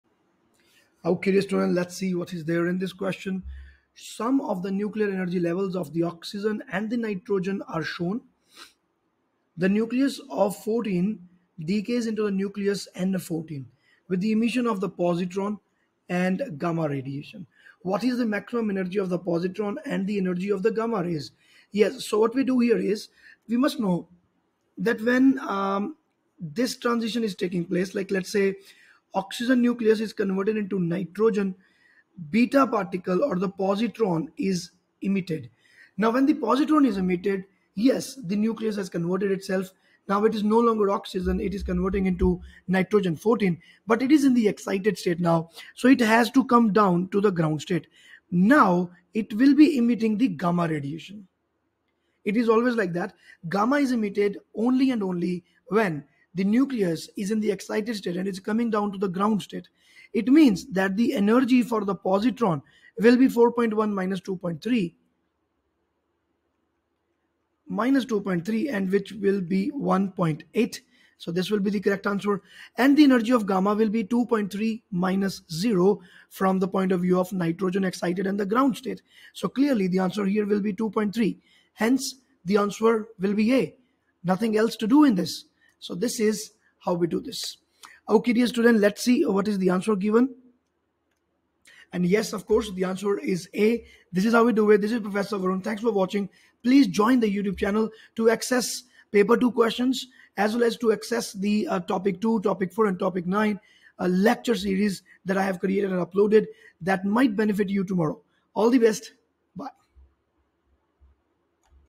Some of the nuclear energy levels of oxygen-14 (14 O) and nitrogen-14 (14 N) are shown. A nucleus of
Some of the nuclear energy levels of oxygen-14 (14 O) and nitrogen-14 (14 N) are shown. A nucleus of 14 O decays into a nucleus of 14 N with the emission of a positron and a gamma ray. What is the maximum energy of the positron and the energy of the gamma ray?